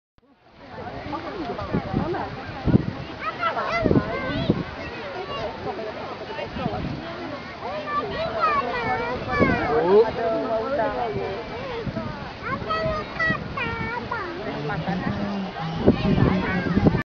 Spinning Kite - Kelantan's International Kite Festival 2013
Spinning Kite at International Kite Festival 2013 locate at Pantai Geting, Tumpat, Kelantan, Malaysia
Really huge kite. Kite was tied at Mitsubishi Pajero's back.